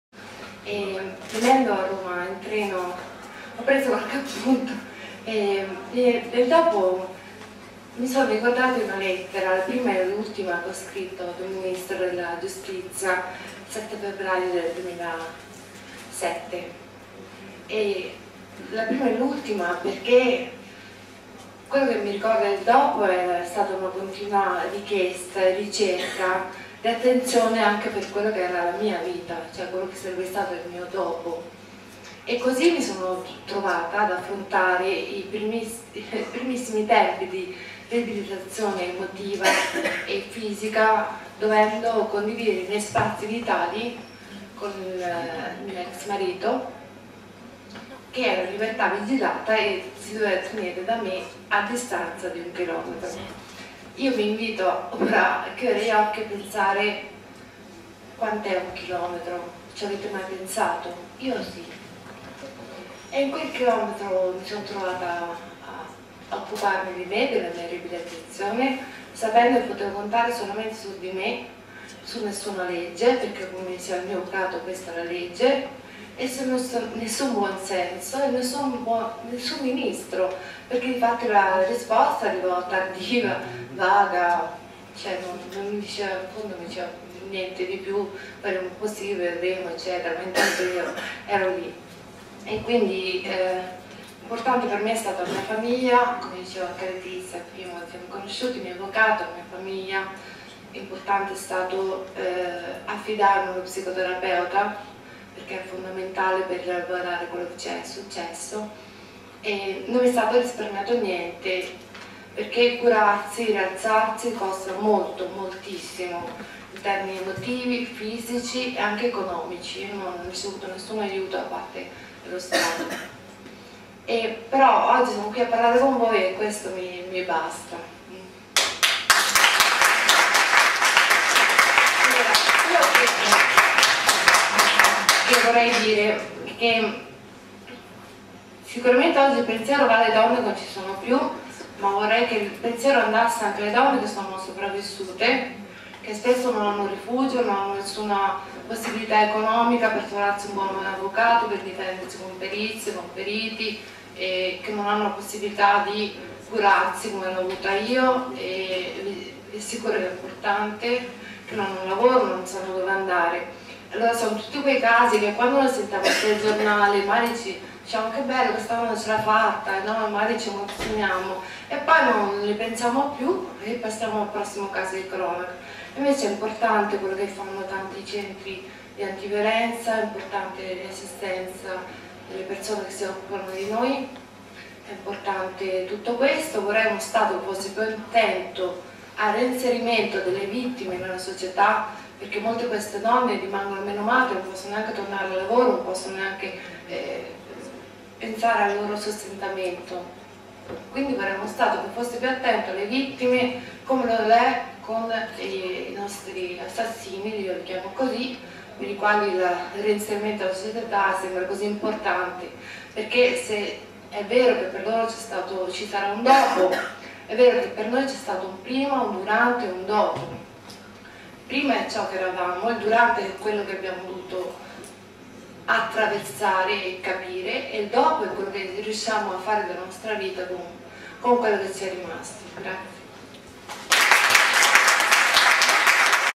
Francesca Baleani.m4v
Francesca Baleani è viva per miracolo!
E' stata vittima dell'aggressione brutale del marito che dopo averla massacrata di botte ha gettato il suo corpo in un cassonetto dell'immondizia.